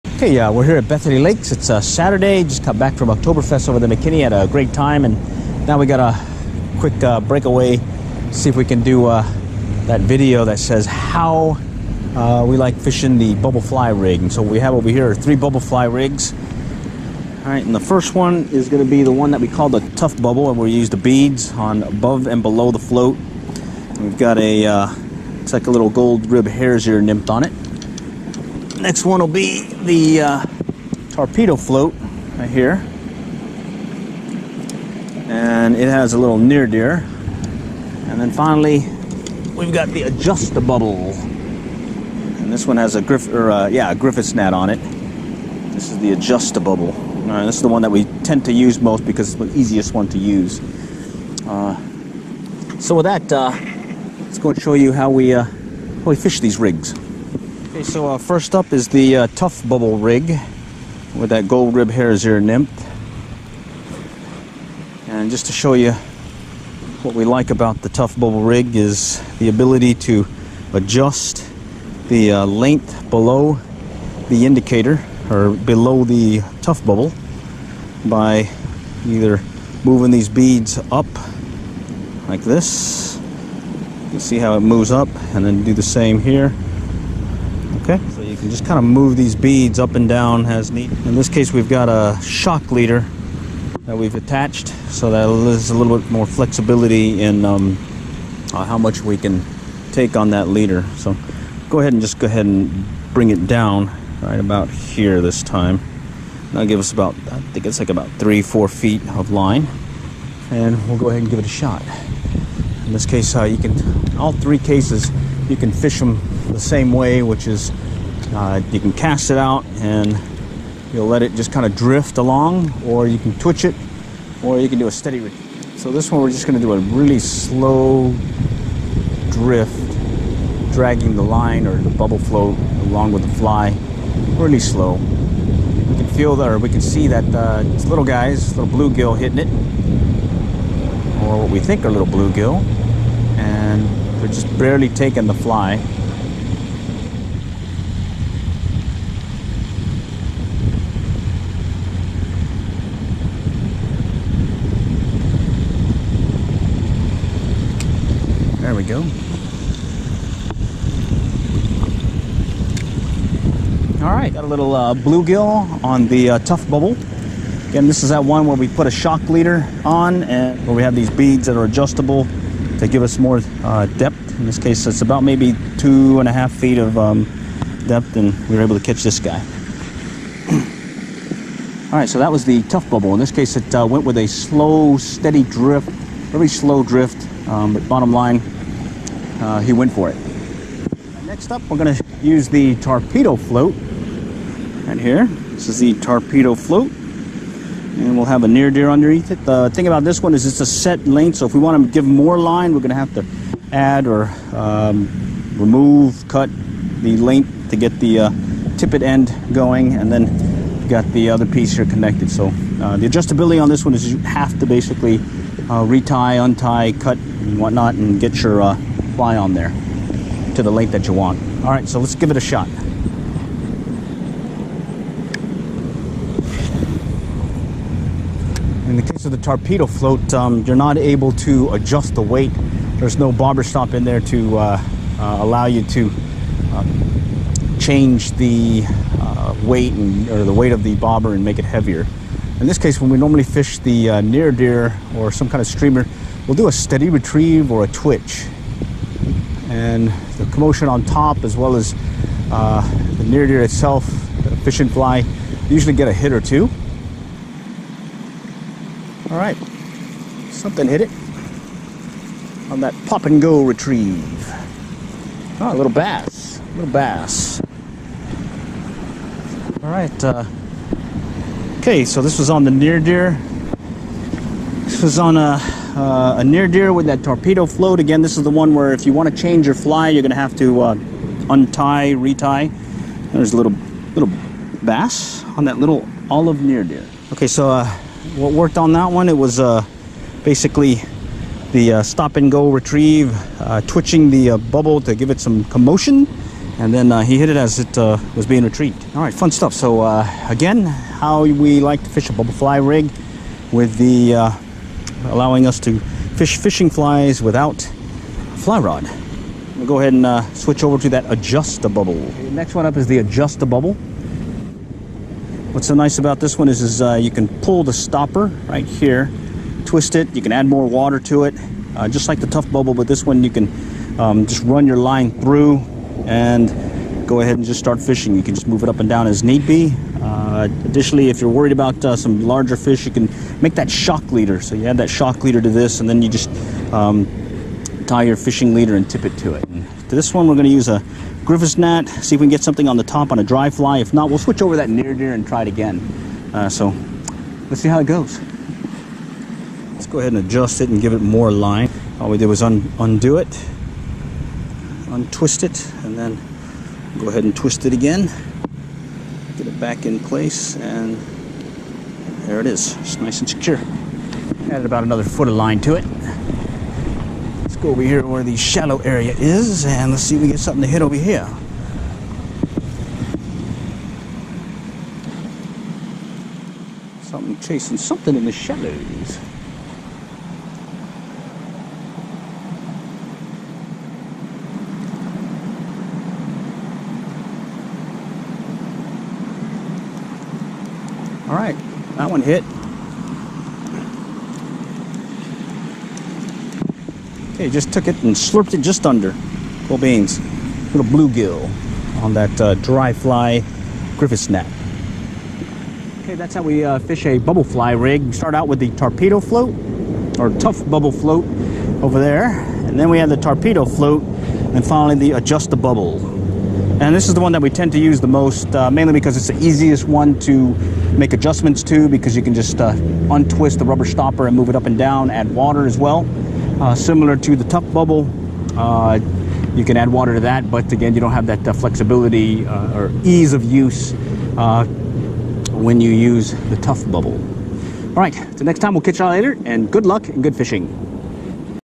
How We Fish Bubble Fly Rigs
Finished the “How We Fish Bubble Fly Rigs” video to go with the article, “How to Set Up Bubble Fly Rigs“! Video shows the three rigs we detailed in the article…

Bubble Float
Torpedo Float
Ajusta Bubble

Lots of fun showing how we fish them; even caught a fish or two! ###
Gear Used
Bubble Fly Rig
A-Just-A Bubble
Lighted A-Just-A Bubble
Plastic Cast Bubble
Torpedo Float